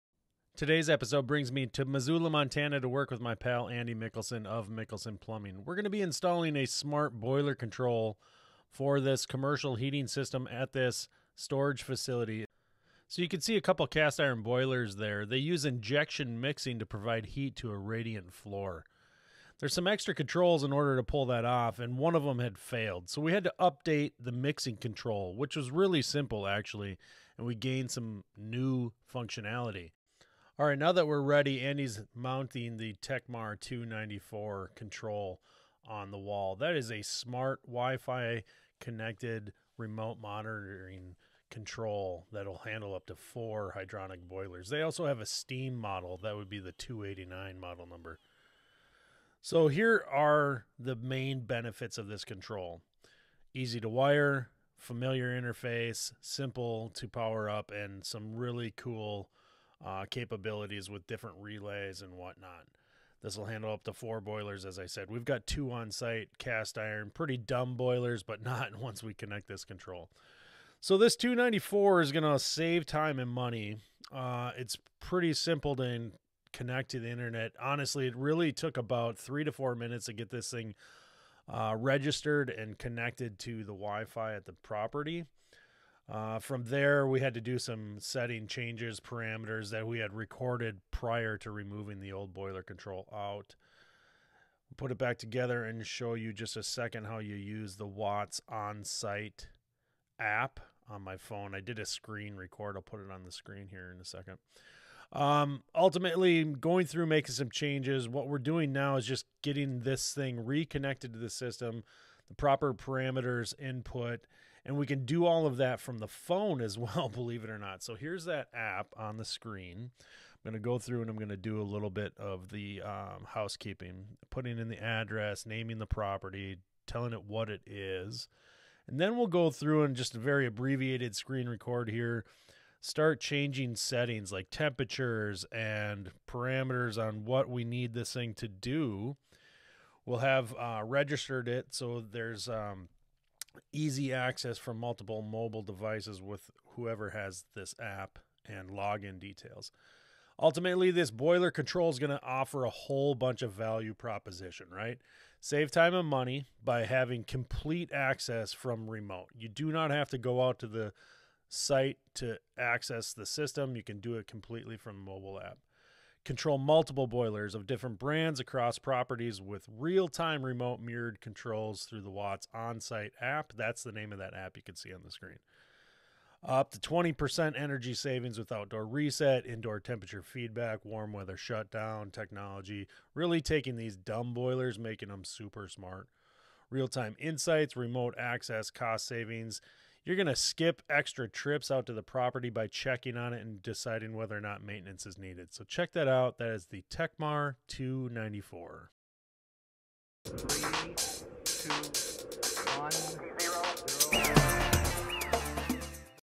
Smart Boiler Control - Tekmar WiFi connected boiler control
Remote monitoring of commercial or residential boilers can off a huge advantage to building monitoring and overall efficiency of maintenance and logistics.

The Tekmar smart boiler controls offer support for multiple boilers both hydronic and steam, all with internet connection via the @WattsWater Onsite mobile app.

For more information about the Tekmar smart boiler controls visit their